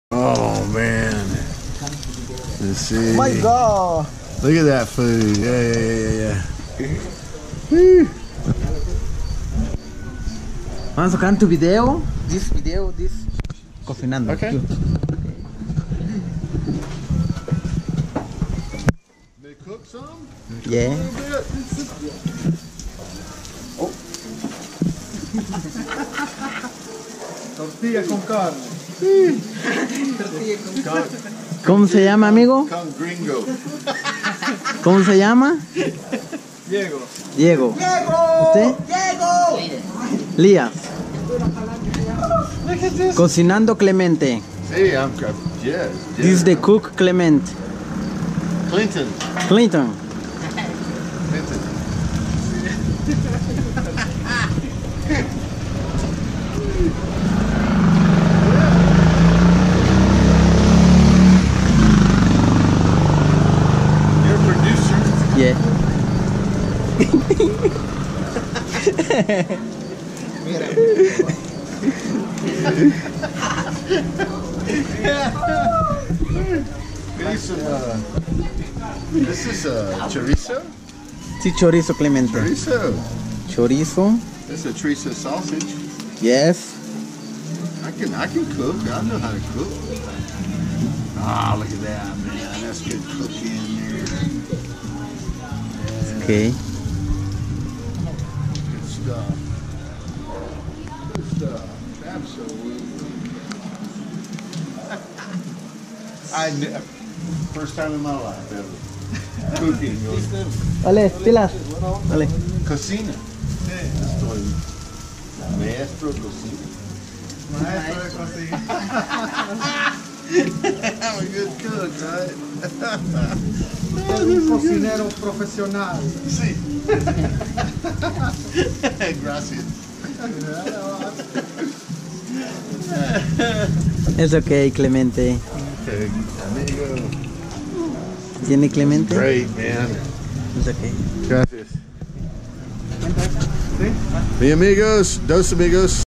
Lake Atitlan, San Pedro Street Cooking
This is El Rancho restaurant  just down the street from the dock.  Excellent food!  I stop by 2 or 3 times a week.  They load me up with some good grub and it's always a blast!  If you are in San Pedro la Laguna you should give them a try.  Menu items are around $4.